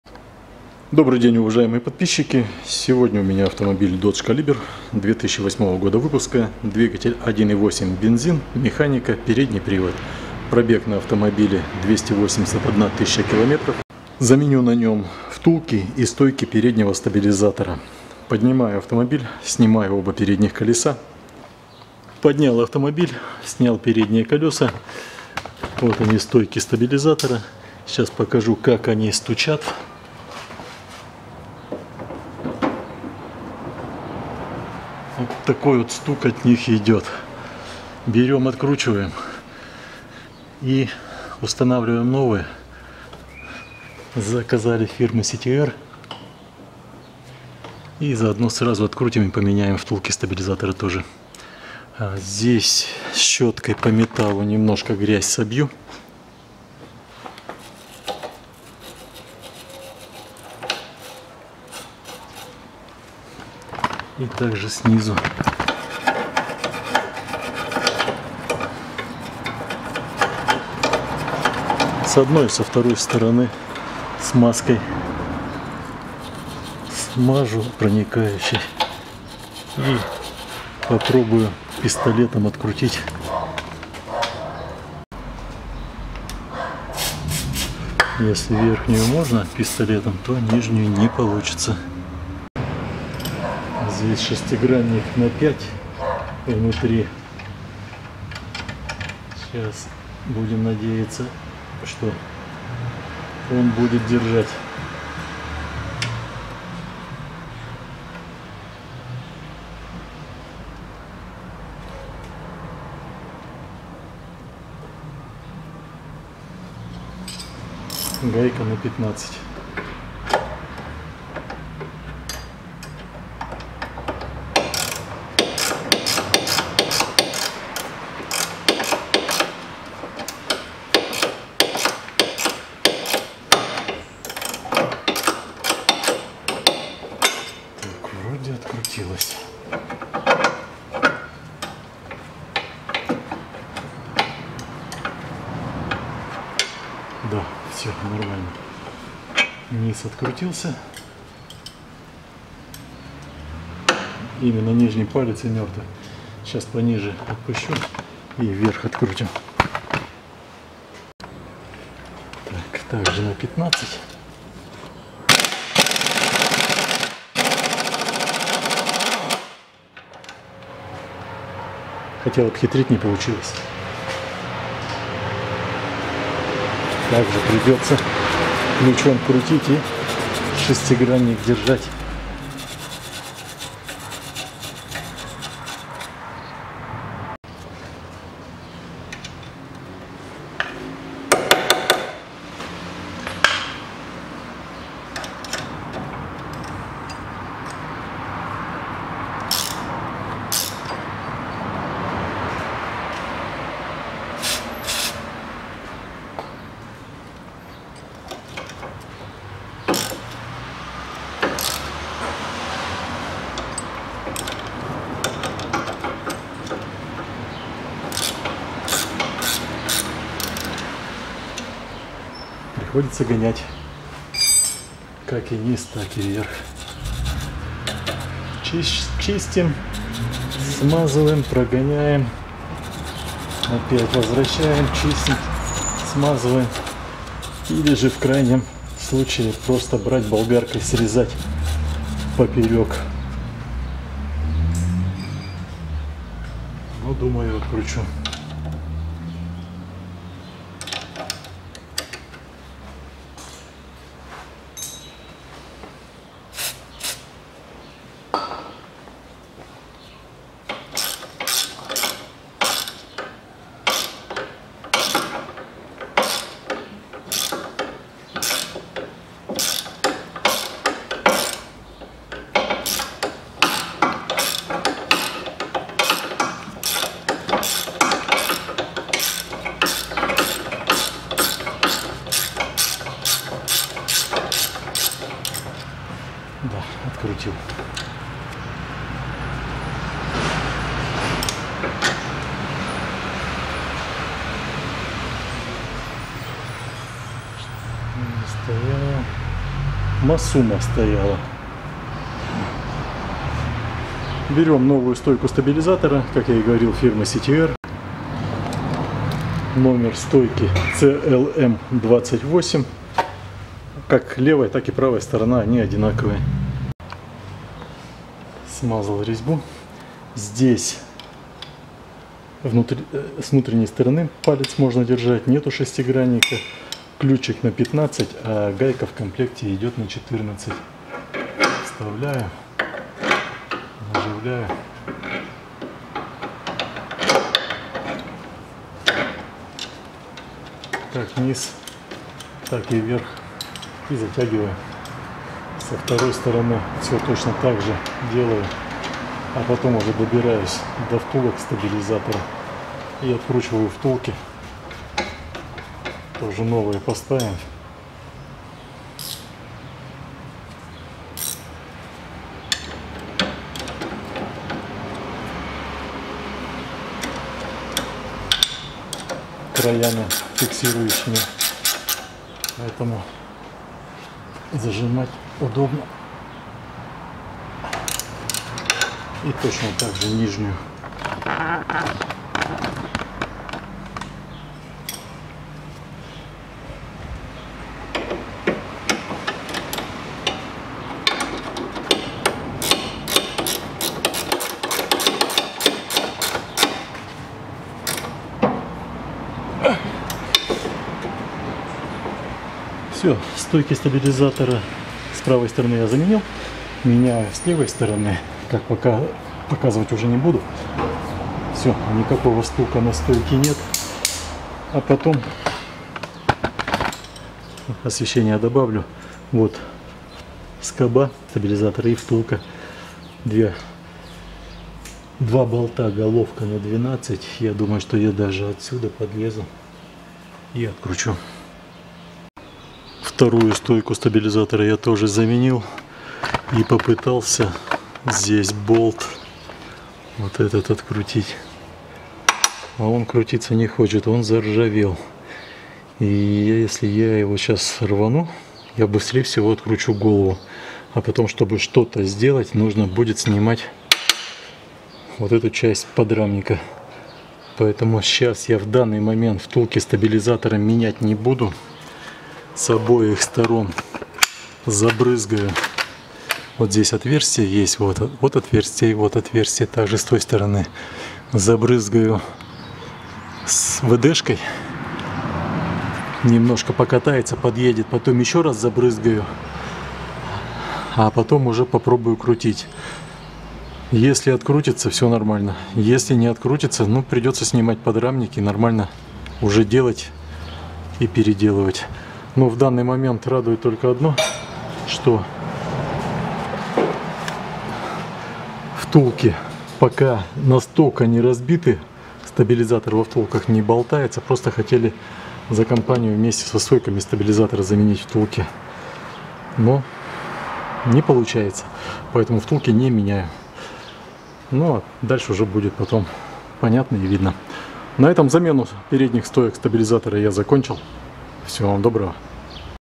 Замена стоек переднего стабилизатора на Dodge Caliber 1,8 Додж Калибер 2008 года
Запчасти для своих нужд заказываю в Краснодаре в магазине TWIN-CAM по т. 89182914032

стойка стабилизатора  CTR  CLM-28